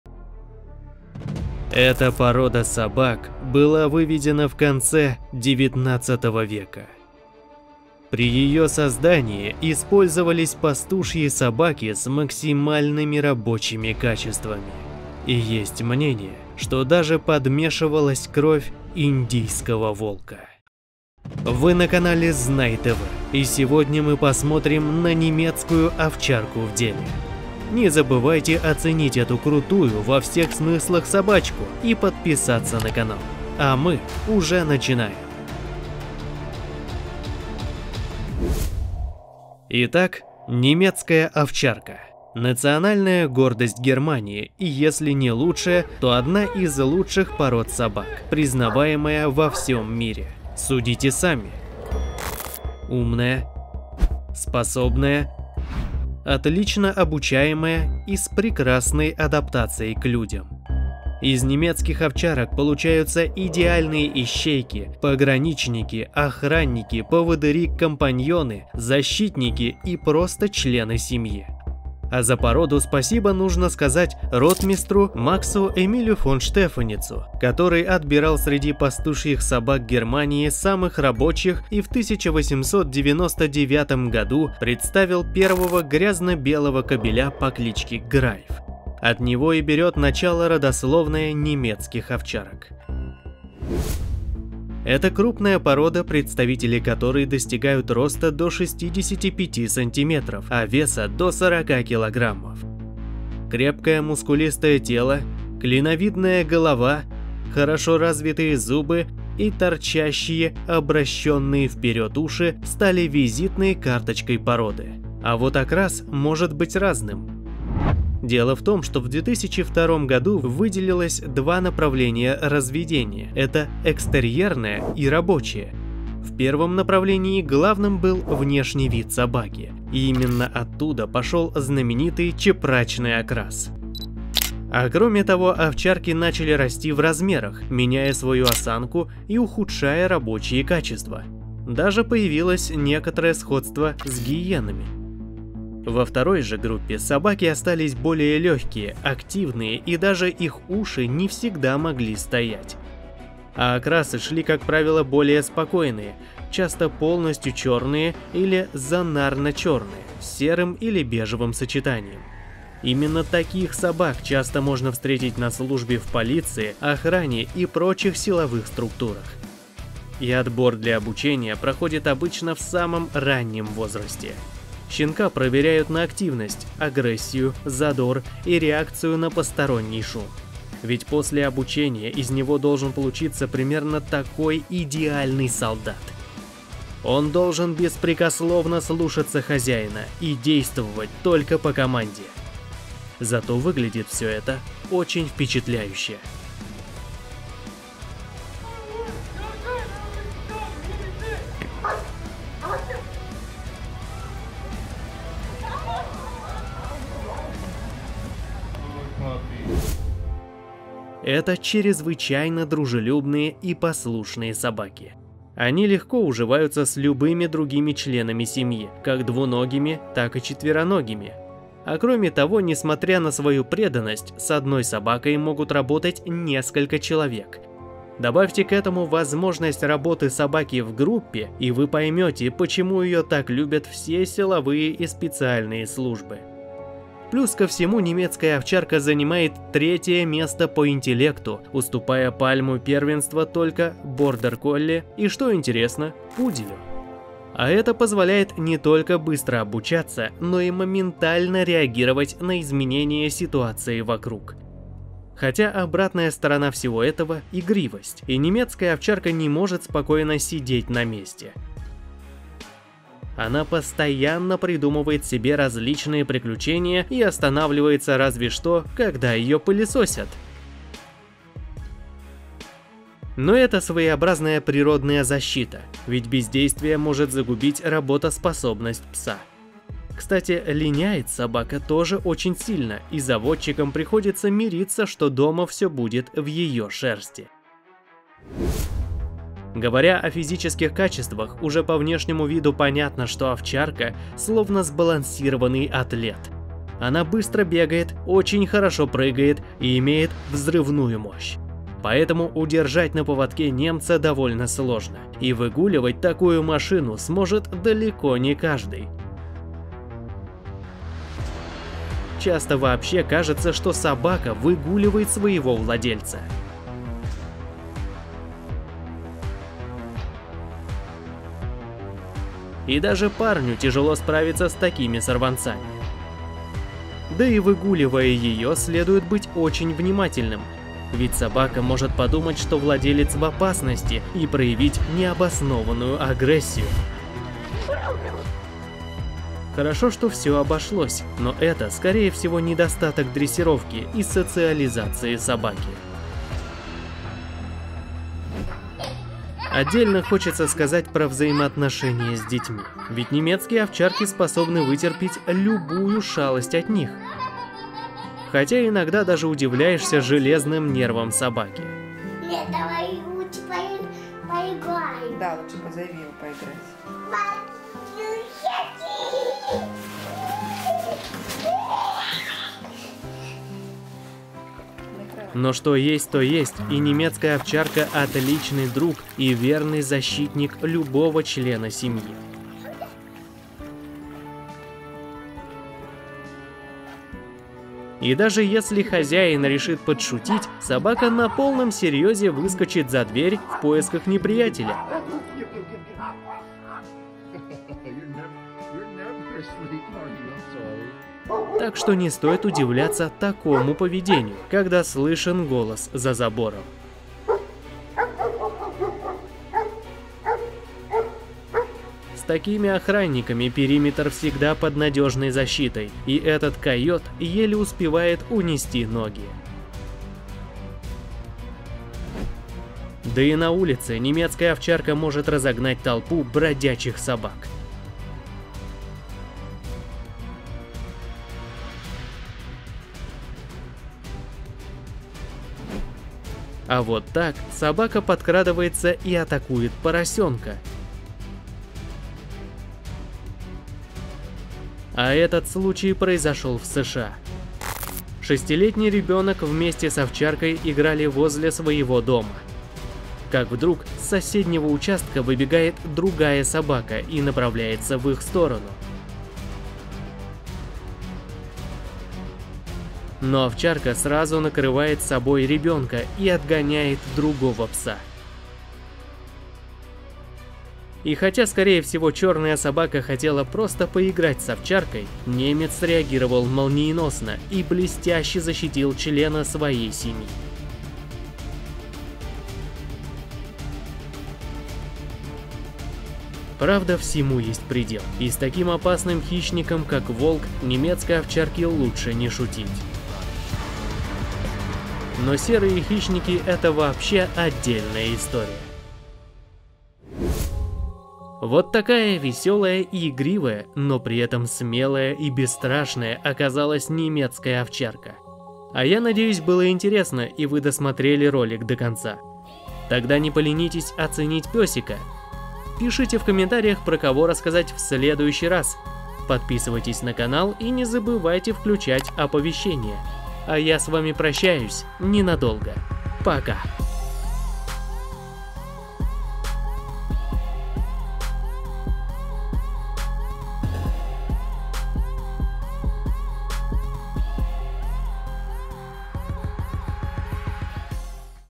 НЕМЕЦКАЯ ОВЧАРКА В ДЕЛЕ! Гроза всех преступников и бесстрашный защитник своего владельца -
Немецкая овчарка - это идеальный боец, который вобрал в себя все самые лучшие свойства! Собаки очень быстрые, ловкие и выносливые, при этом имеют мощный хват и очень сильные! А этого достаточно, чтобы остановить любого нарушителя или вора!
Кроме того овчарка занимает 3е место в списке самых умных пород, она отлично обучается и способна работать в команде! Поэтому все силовые структуры с большим удовольствием используют немецких овчарок! Да и для семьи это верный друг, который никогда не даст ребенка в обиду, спасет его из воды и защитит от нападения других собак или людей!
В общем очень крутая порода, хотя и со своими минусами!